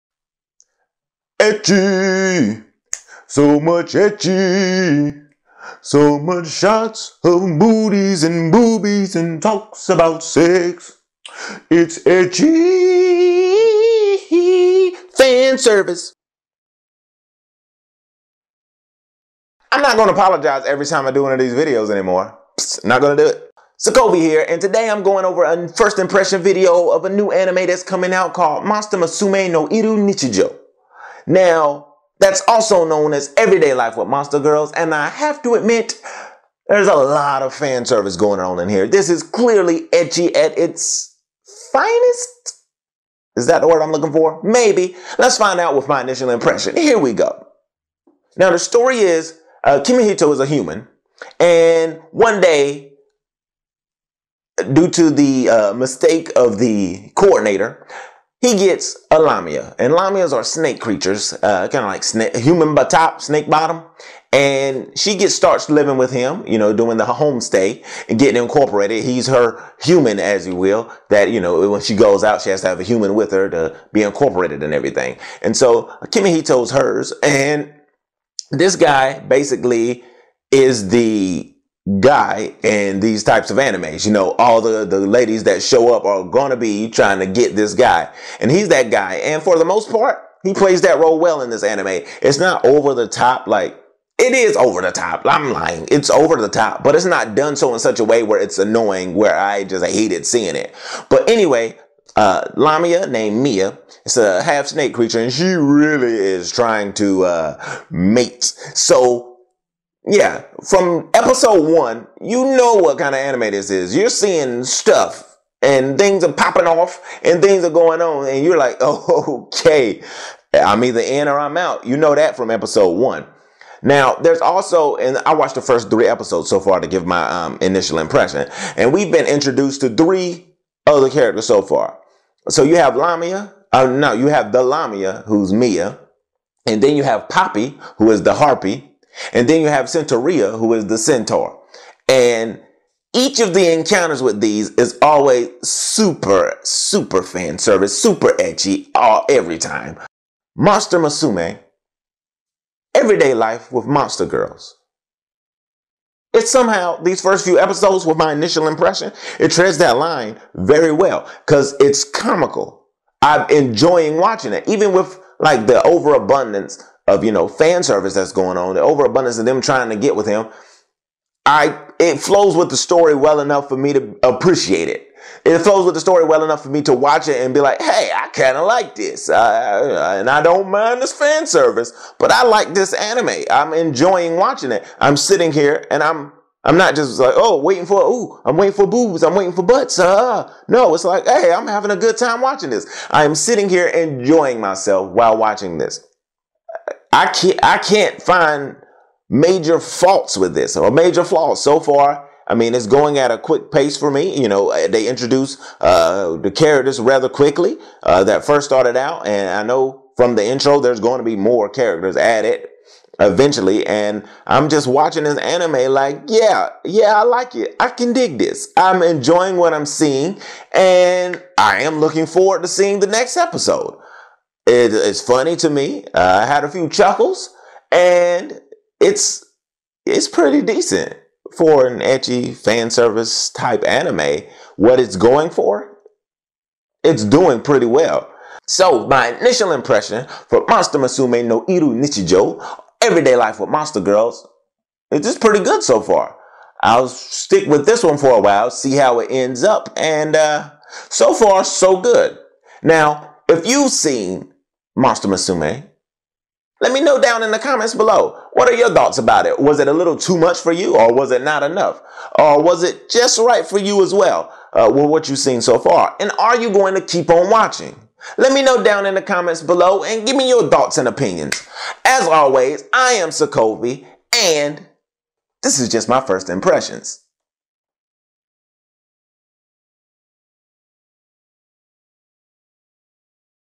Monster Musume No Iru Nichijou - Everyday life with Monster Girls - Review First Impressions
Boobs, Booty, and talking Sex... But it be ecchi on the screen, No hentai here.

モンスター娘のいる日常